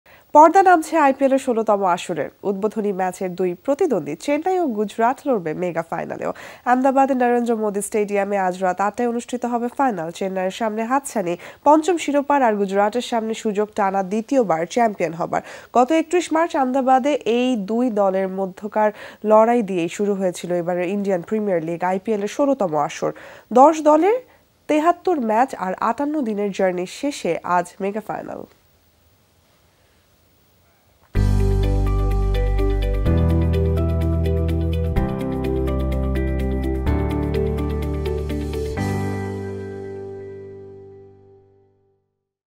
আইপিএলের ফাইনাল আজ; শিরোপা উঠবে কার হাতে? | IPL | Jamuna Sorts
পর্দা নামছে আইপিএলের ১৬তম আসরের। উদ্বোধনী ম্যাচের দুই প্রতিদ্বন্দ্বী চেন্নাই ও গুজরাট লড়বে মেগা ফাইনালেও। আহমেদাবাদের নরেন্দ্র মোদি স্টেডিয়ামে আজ রাত ৮টায় অনুুষ্ঠিত হবে ফাইনাল। চেন্নাইয়ের সামনে হাতছানি পঞ্চম শিরোপার আর গুজরাটের সামনে সুযোগ টানা দ্বিতীয়বার চ্যাম্পিয়ন হবার । গত ৩১ মার্চ আহমেদাবাদে এই দুই দলের মধ্যকার লড়াই দিয়েই শুরু হয়েছিল এবারের ইন্ডিয়ান প্রিমিয়ার লিগ আইপিএলের ১৬তম আসর। ১০ দলের ৭৩ ম্যাচ আর ৫৮ দিনের জার্নি শেষে আজ মেগা ফাইনাল।